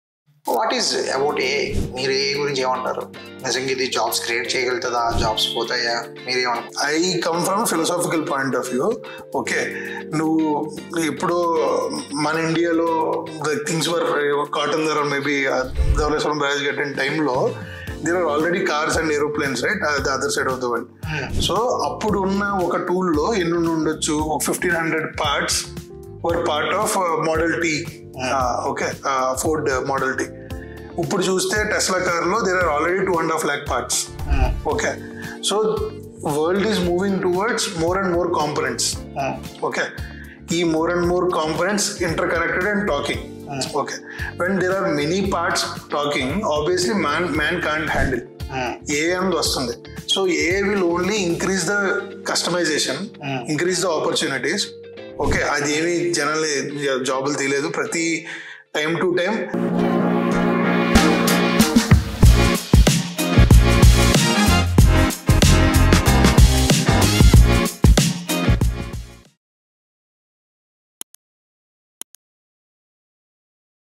AI Is Not Magic — It’s a Solution to Human Limits
What is AI? Will jobs disappear or continue?
Senior Product Manager Ravi R.S. Nadimpalli answers with a brilliant perspective:

He explains that the world keeps getting more complex.
In 1870, India was building the Dowleswaram Barrage — while in other parts of the world, cars and airplanes were evolving.
A Ford Model T had 1,500 parts, but today’s Tesla has 2.5 lakh components — all interconnected and constantly communicating.

As the number of components increased, human ability to handle everything decreased.
That’s why AI came — not to replace humans, but to manage complexity humans can’t.

🎙 From Yuva Vikash – Expert Talks Series
👤 Guest: Ravi R.S. Nadimpalli
🎤 Host: Prem Chand